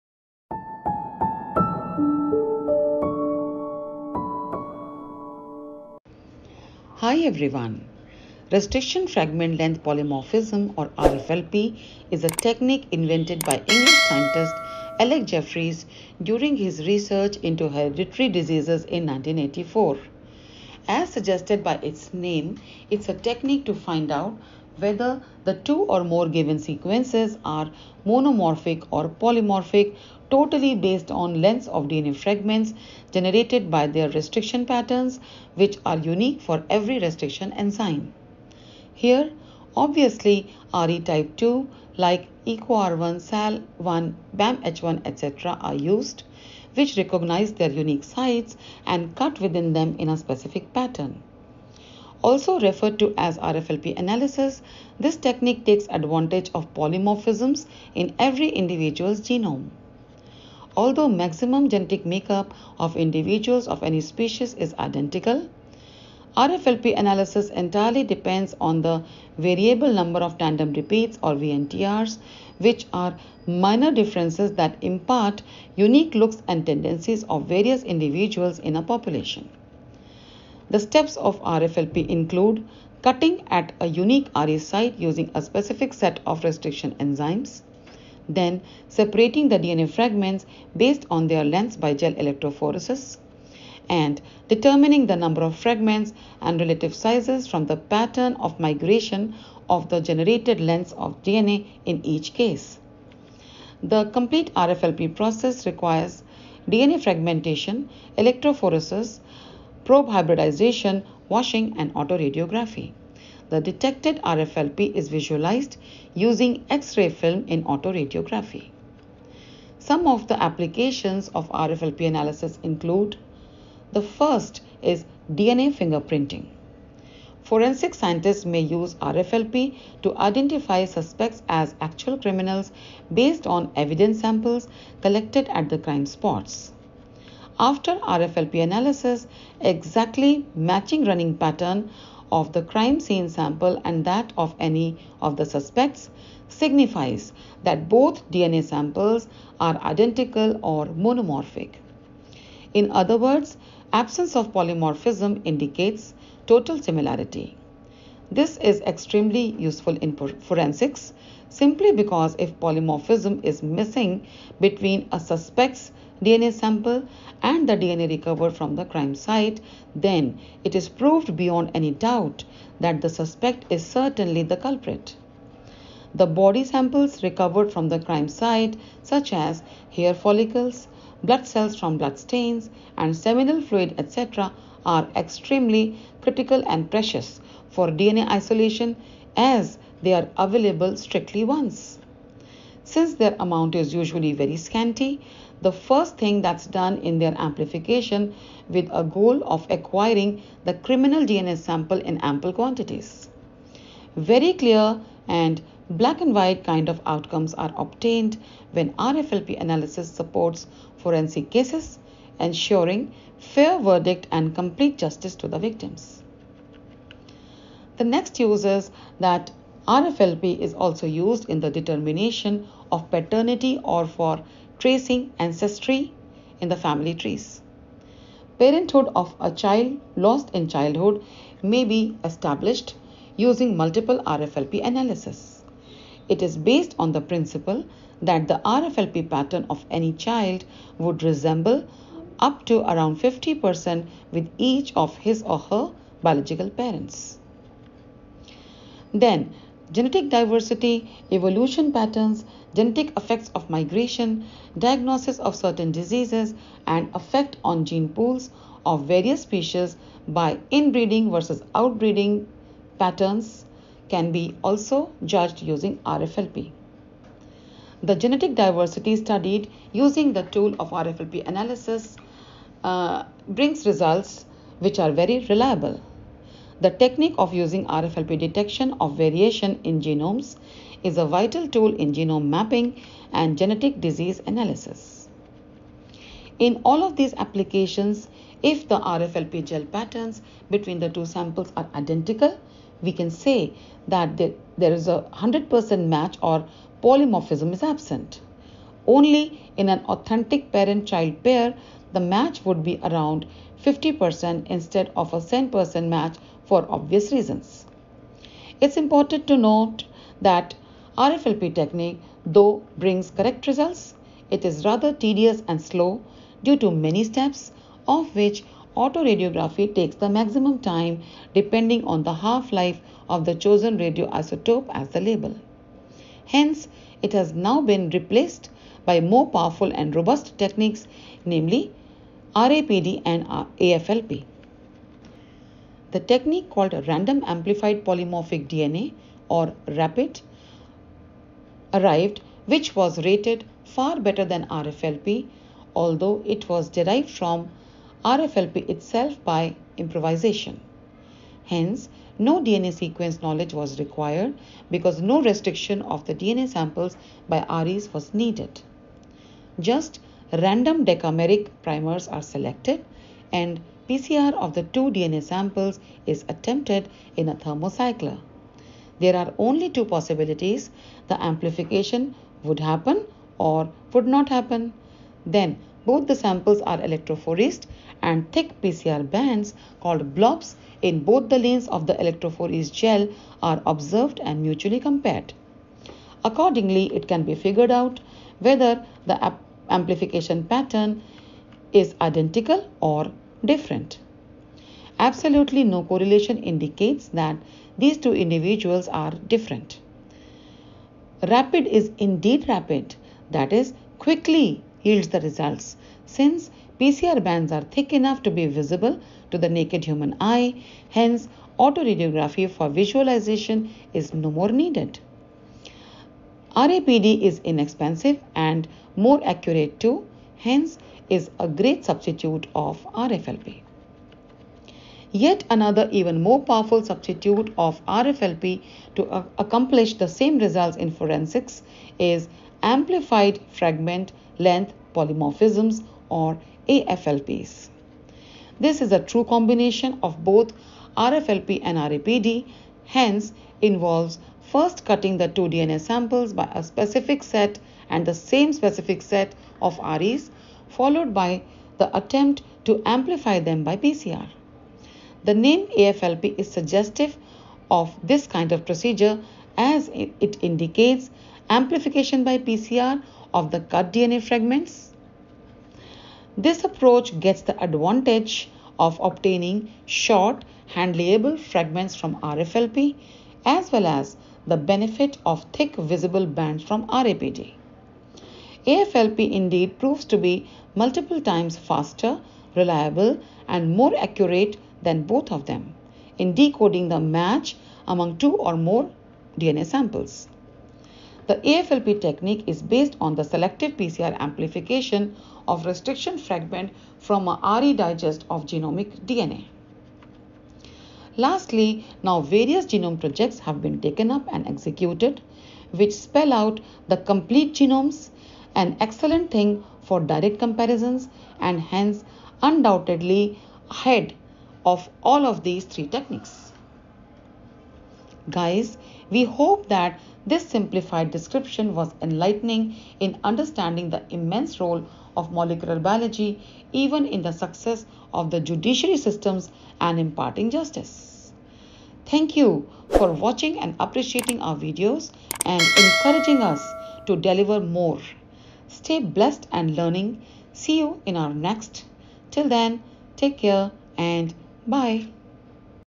Application of RFLP (Restriction Fragment Length Polymorphism) in Forensics
Brahmjeet Dahiya & Archana Ayyagari explain to their worthy audience the principles of RFLP, RAPD & AFLP in an oversimplified manner, just like a cakewalk to all the biology and law enthusiasts. Gripping and very easy to follow, these simple yet powerful techniques have proved to be a boon to the judiciary system for imparting fair verdict and justice to victims of any kind of molestation. This is carried out utilizing any body samples left behind as a result of the struggle between the victim & the molester at the crime scene. Identifying the criminal based on these techniques is rather foolproof and satisfactory.
Immense thanks to our dedicated YouTube family for great viewership and encouragement.